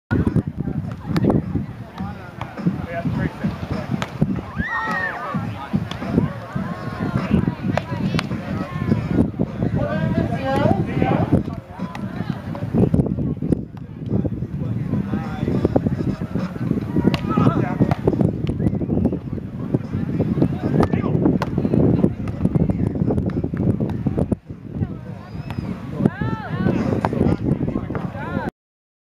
Live 2deep Sports - HBO 1405
7/23 & 7/24 -- Harbor Beach Open -- Men's & Womens Open -- Santa Cruz, CA
Supporting the Santa Cruz California sports community!